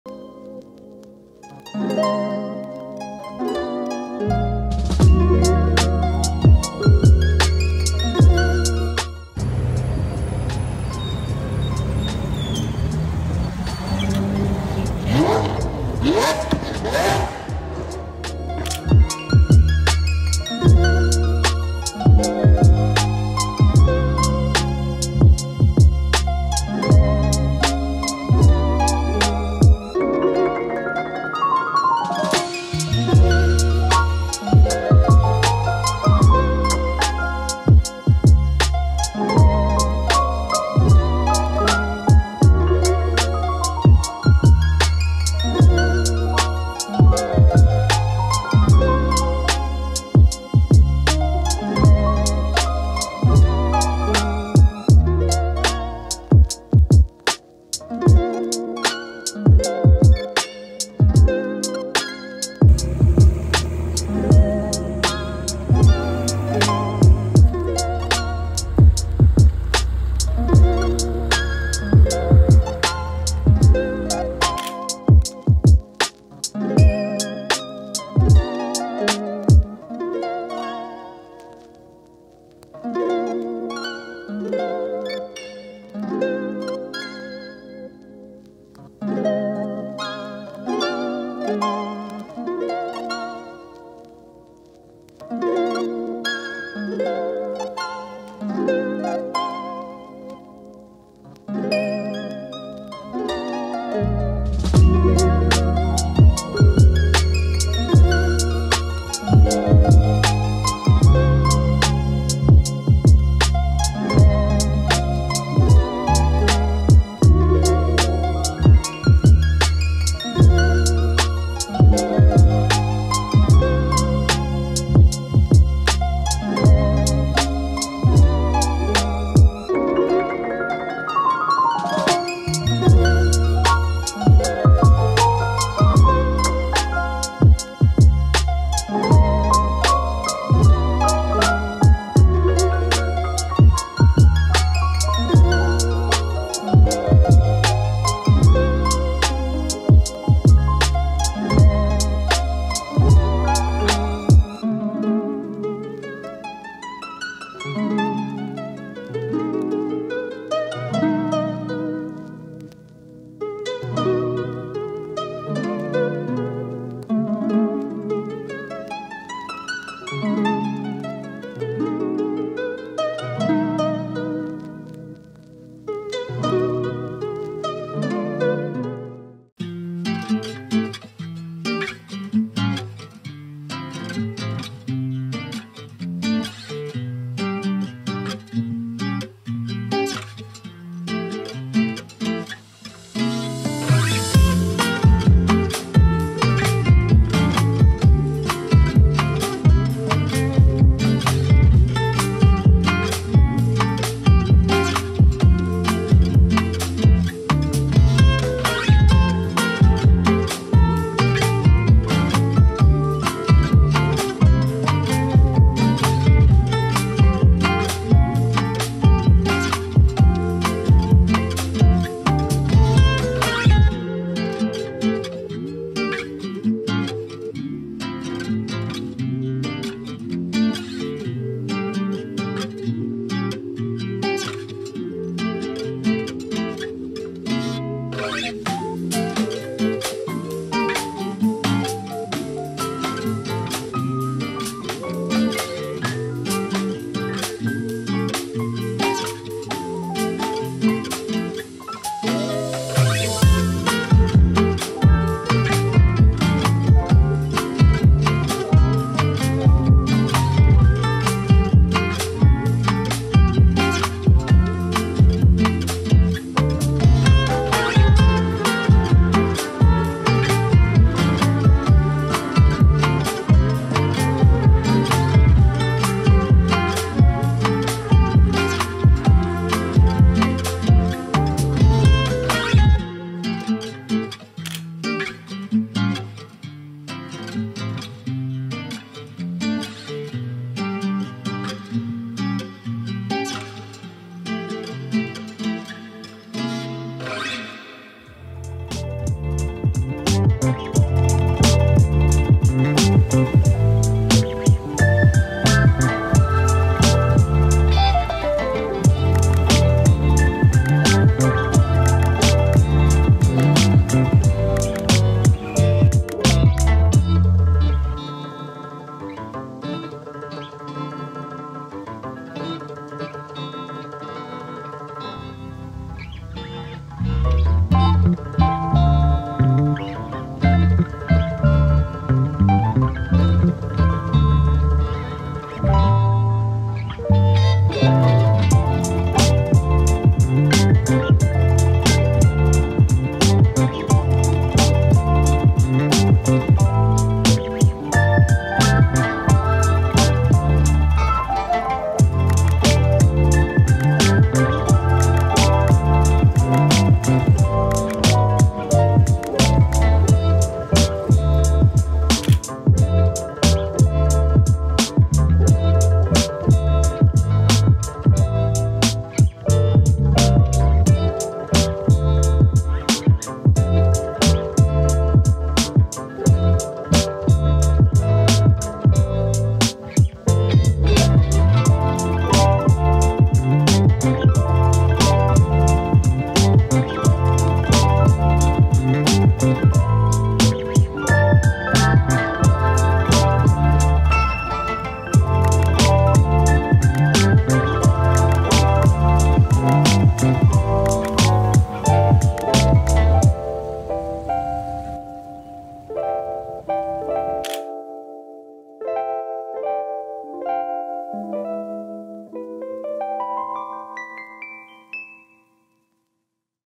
Car Spotting in Manila, Philippines - Bonifacio Global City (May 2024)
A trip to the other side of the world to explore the Philippines and check out the car culture! There was 3 main areas I checked out

- Bonifacio High Street area
- Karrera Showroom
- The Trans Sport Show at SMX Convention Center Manila

I saw a huge amount of different cars which I have not seen in the UK such as a GMC, Various highly modified JDM cars and Mini vans with TE37 wheels!

Instagram and Tik-Tok: Jamesinshot